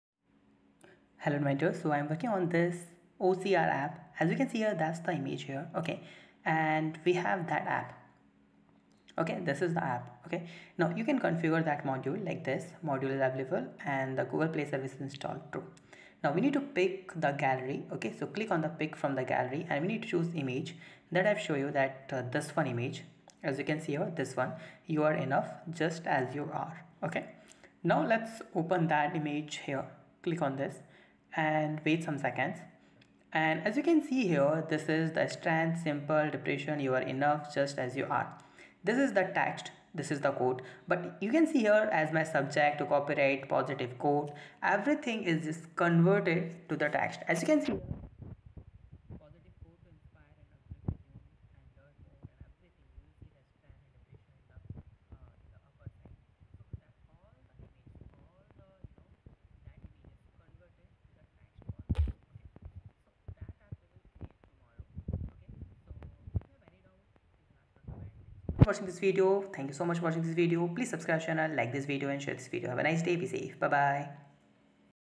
Convert Image To Text App in MIT App Inventor 2 | OCR App New
In this informative video, I demonstrate how to create an app using MIT App Inventor 2 that converts images to text using OCR. Whether you need to digitize printed documents, capture text from signs, or extract information from images, this app provides a convenient solution. Join me as I walk through the step-by-step process of building and testing this useful app, showcasing the capabilities of MIT App Inventor 2 and the exciting possibilities it offers for mobile app development.